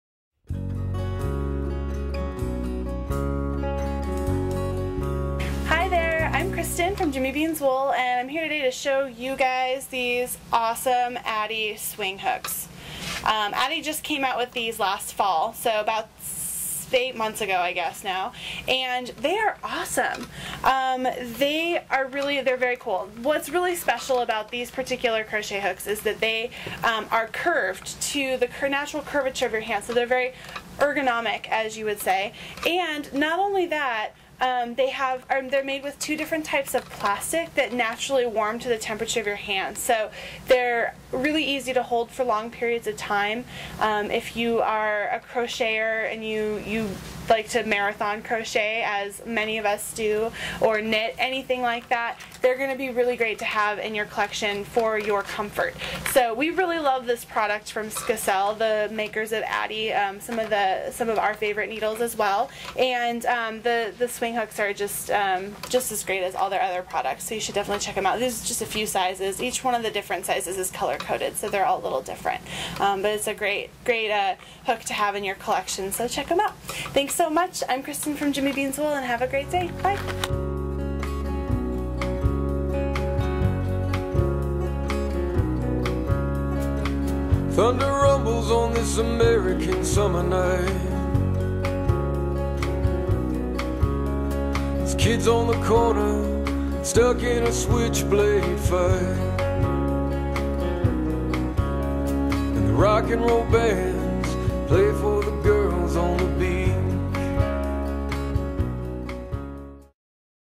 Addi Swing Hooks Review
Kristen from Jimmy Beans Wool reviews Addi Swing Hooks.  Jimmy Beans Wool is your LYS... online!